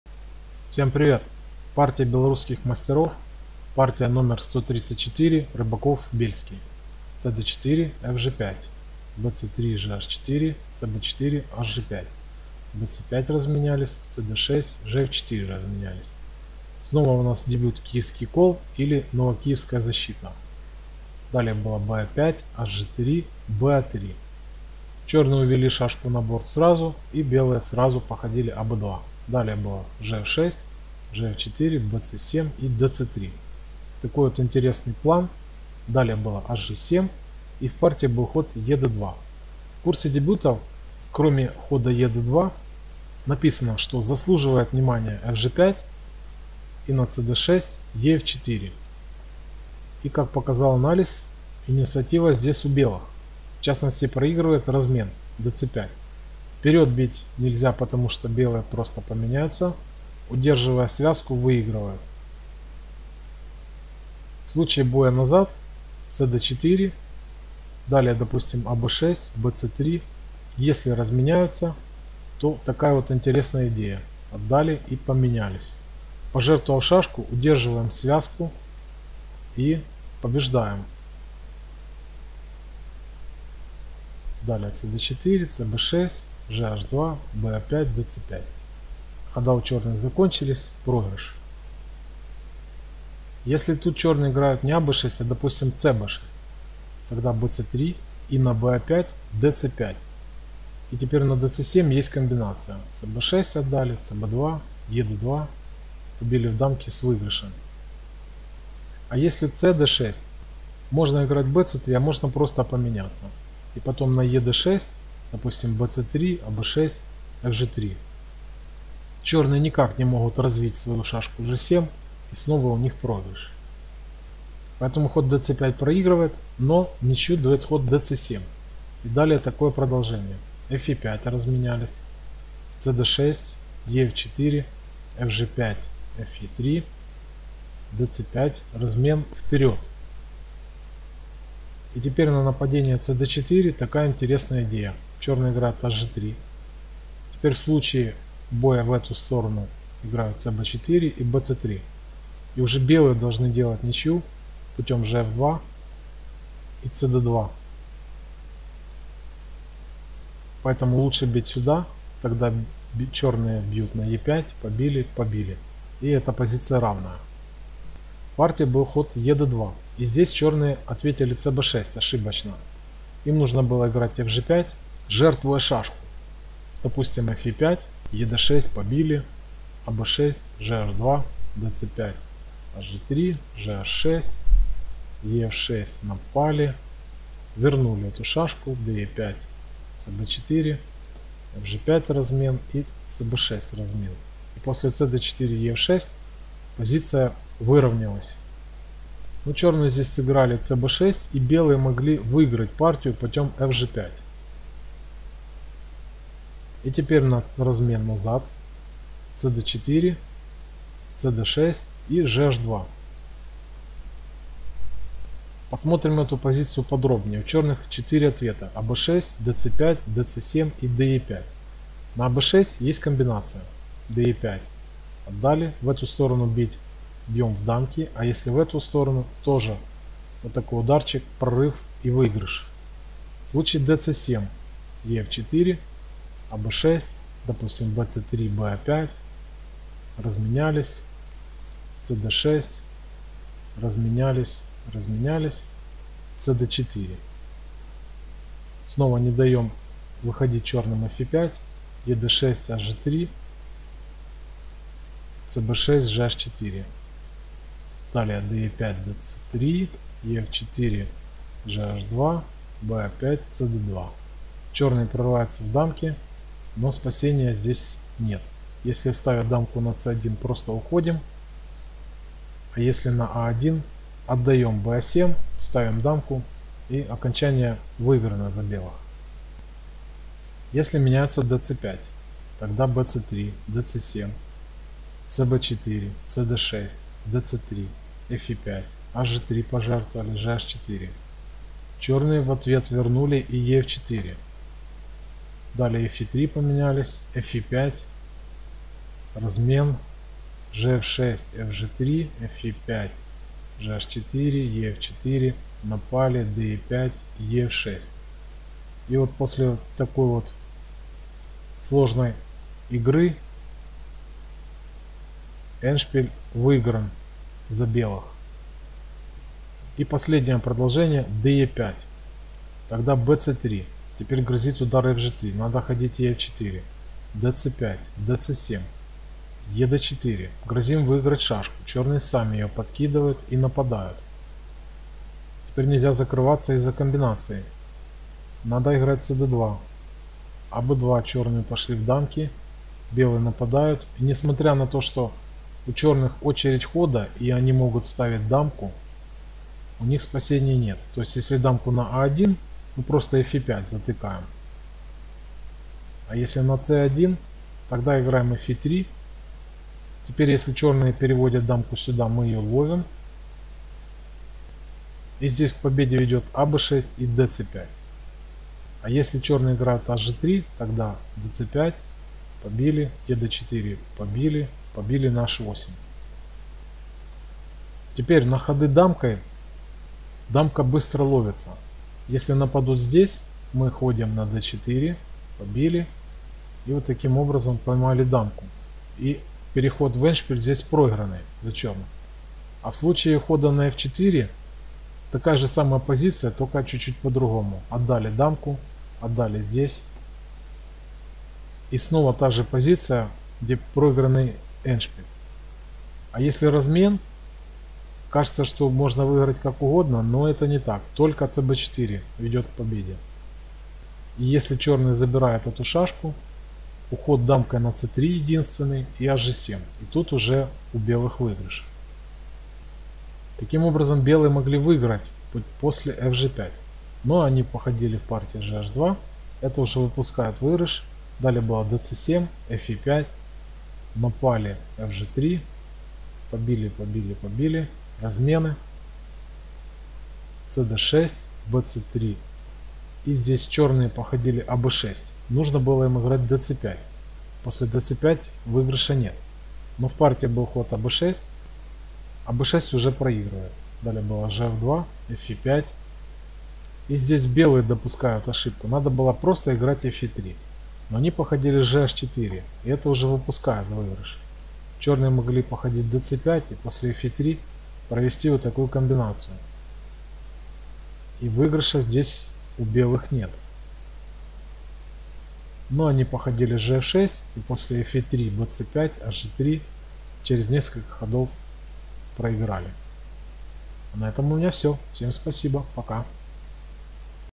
Шашечные партии белорусских мастеров (Партия № 134)
ссылка на это видео:
Ищем упущенные возможности в книге "Шашечные партии белорусских мастеров "
,авторы Б.Л.Берлинков , А.В.Рокитницкий ,Минск,1983г.
00:00 Партия №134

полезные ссылки:
-сайт для игры в шашки
-сайт Гамблер ,клуб умных игр